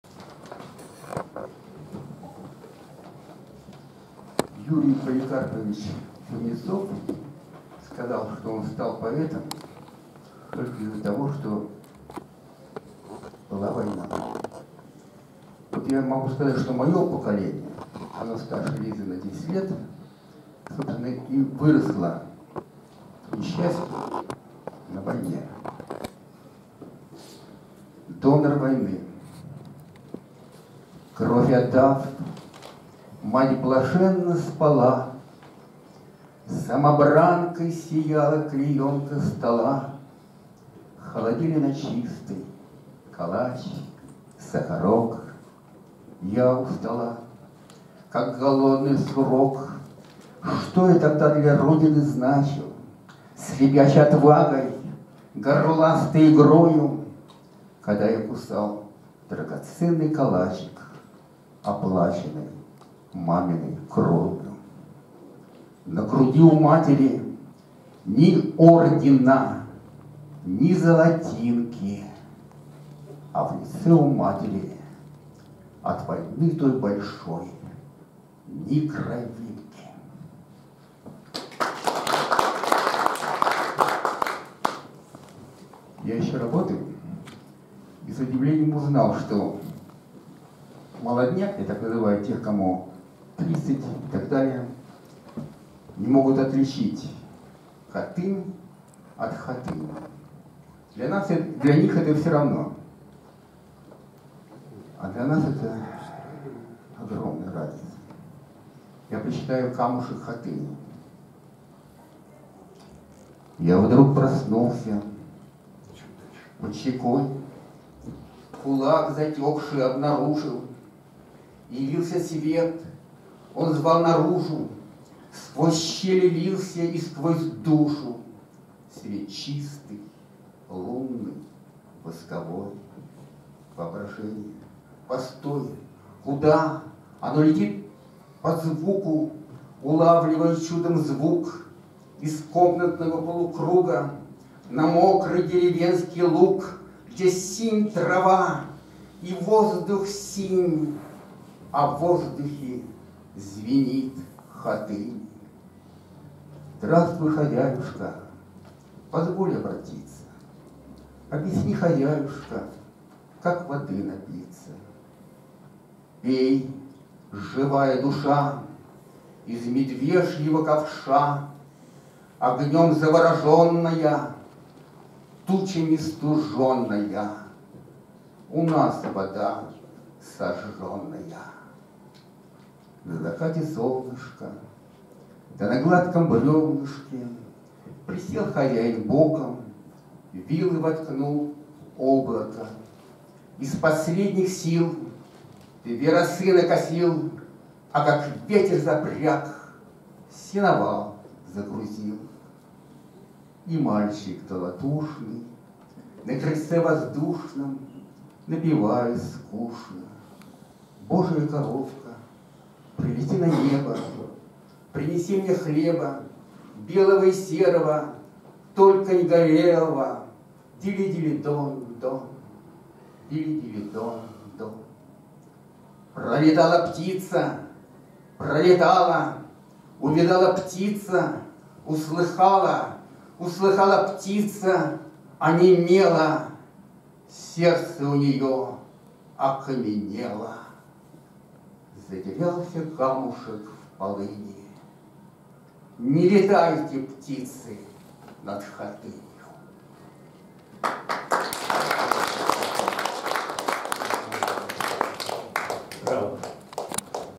Герман Беляков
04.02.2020 Клуб "Мелодия стиха" Центральный Дом Литераторов (ЦДЛ), ул. Большая Никитская, 53 Москва